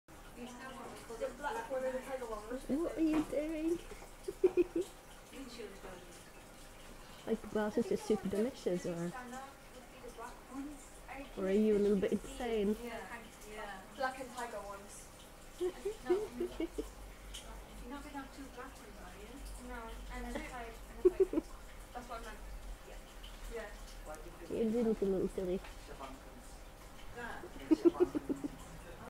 Silly, silly hamster.
Currently for sale at creature company in Wimbledon. I may go back and buy her.